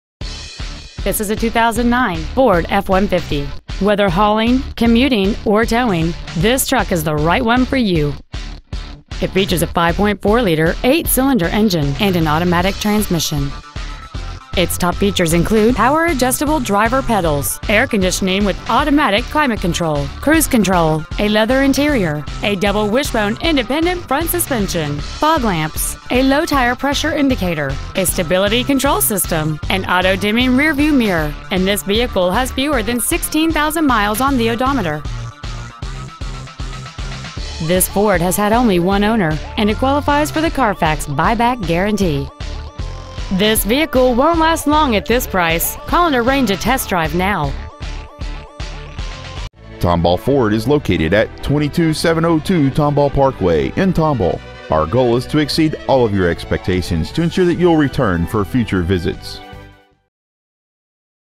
2009 Ford F-150 Tomball TX
Year: 2009
 Make: Ford
 Model: F-150
 Engine: 5.4L 3V EFI V8 FFV ENGINE
 Trans.: automatic
 Exterior: Oxford White
 Miles: 15,430
 Interior: Tan
 Stock: